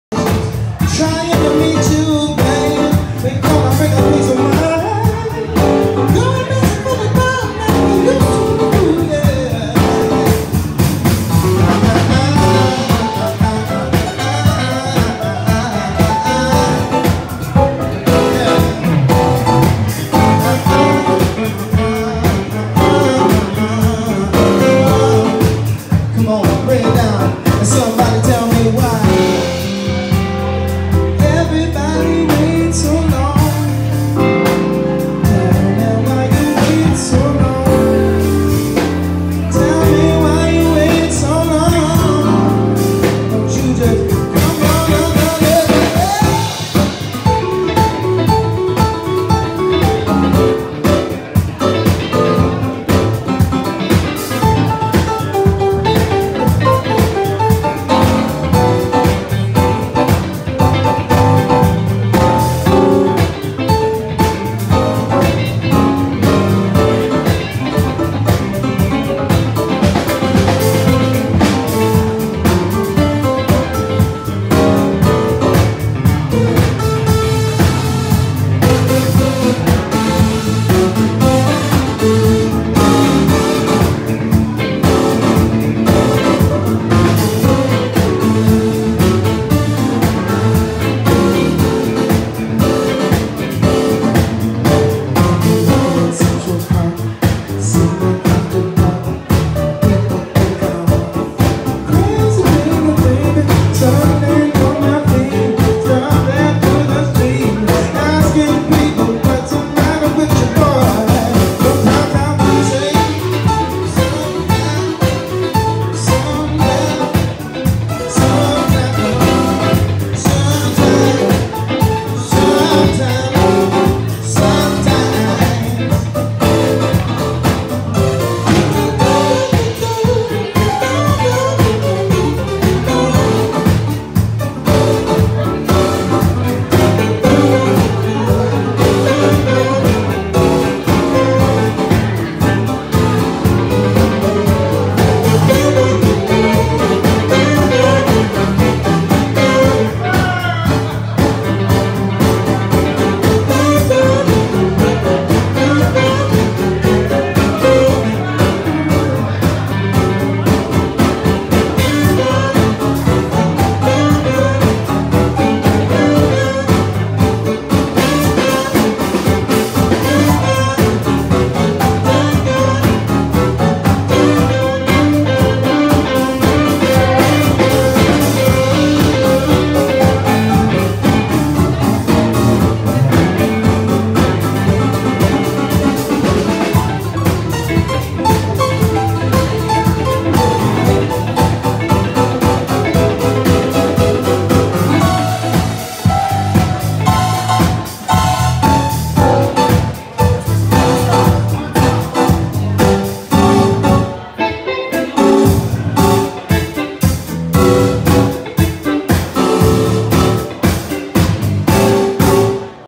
Dancing #2
Another short video of me dancing... the band playing is
Ezra & the Relatives...